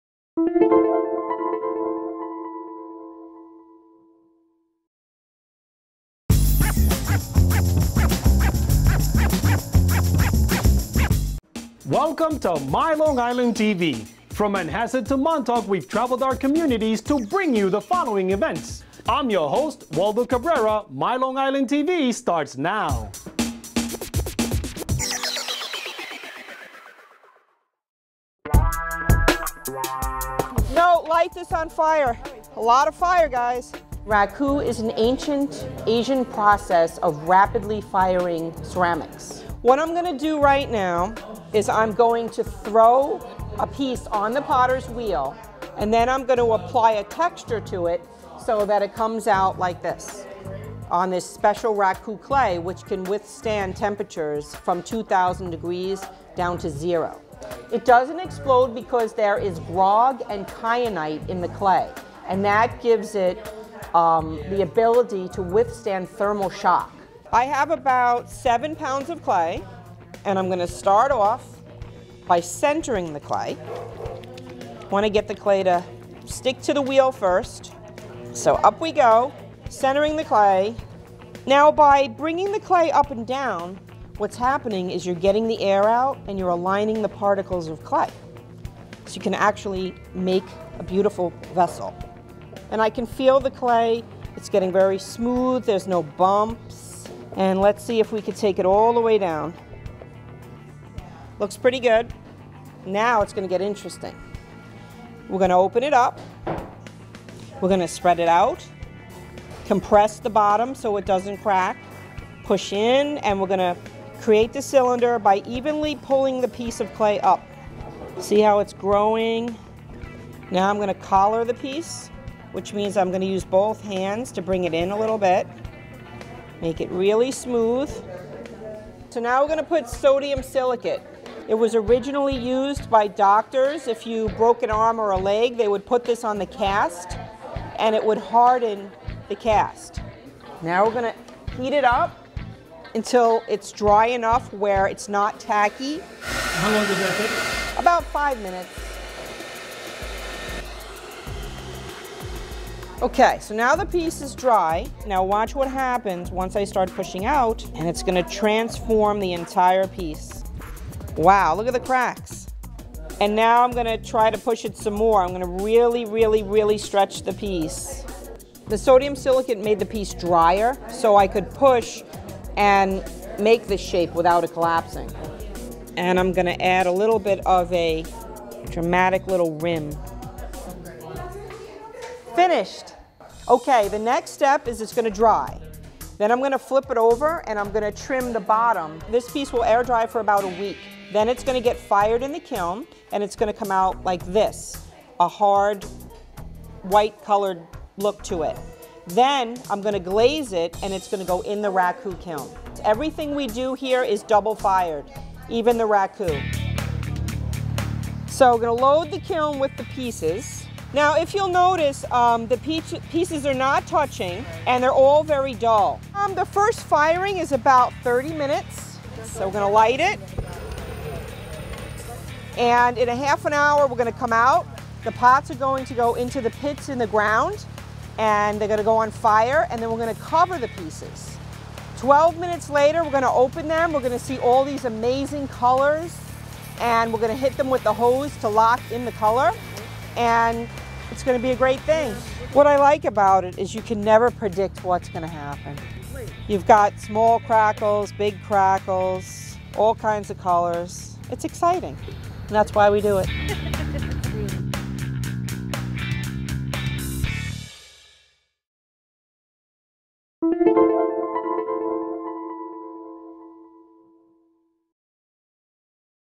How to do Raku Pottery with Gina Mars at the Art League of Long Island
Video Journalist Waldo Cabrera

Gina Mars, Raku Instructor at the Art League of Long Island demonstrates the art of Raku

Featured Interview:
Gina Mars